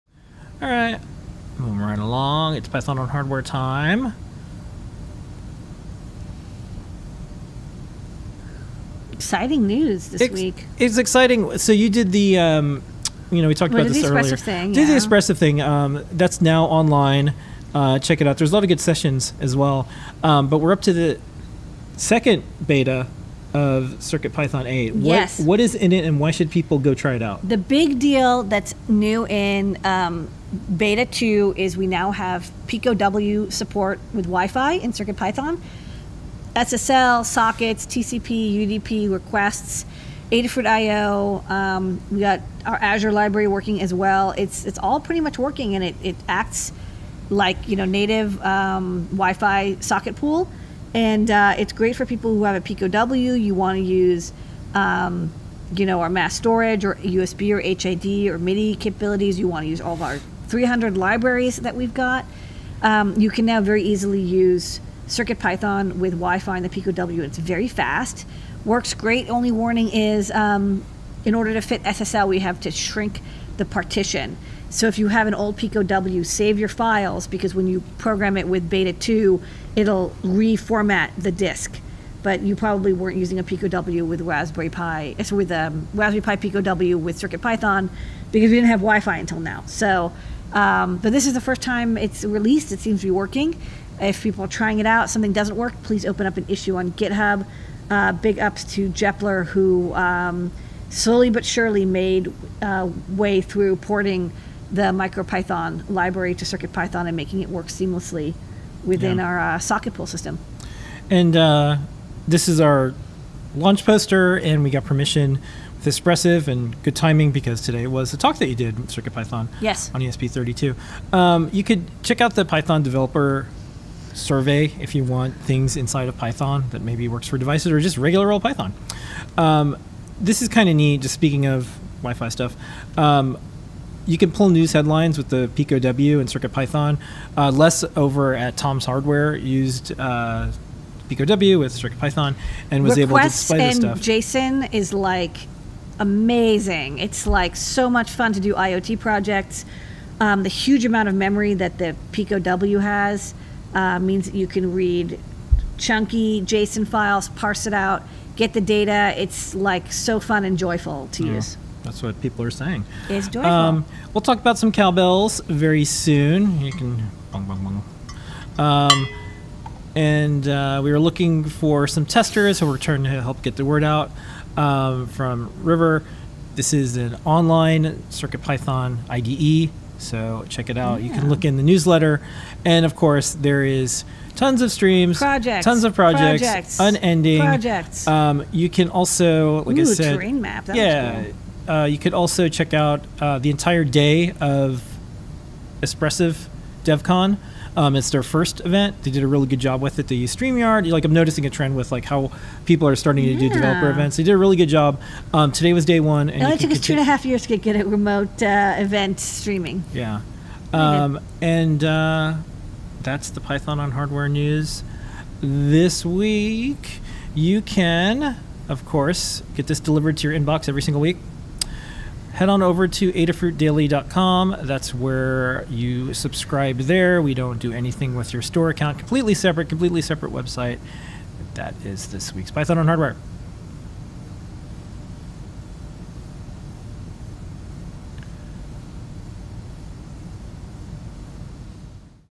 Python on Hardware weekly video 202!
The wonderful world of Python on hardware! Episode 202 (October 19, 2022). This is our weekly Python video highlights! Ladyada and PT review the Python on hardware news of the week.